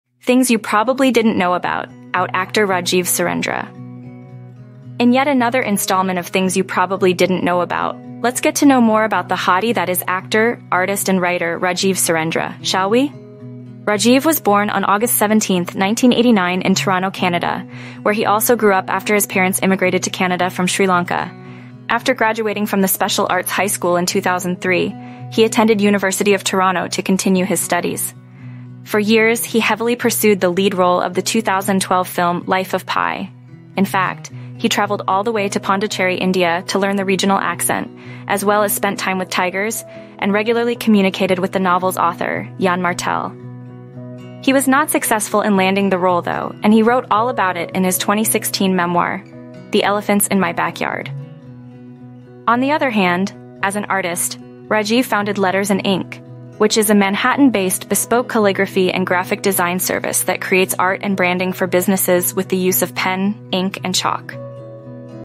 Things You Probably Didn’t Know About Out Actor Rajiv Surendra
Rajiv was born on August 17, 1989 in Toronto, Canada, where he also grew up after his parents immigrated to Canada from Sri Lanka. He was a musical theater student and performer at Wexford Collegiate School for the Arts. After graduating from the special arts high school in 2003, he attended University of Toronto to continue his studies.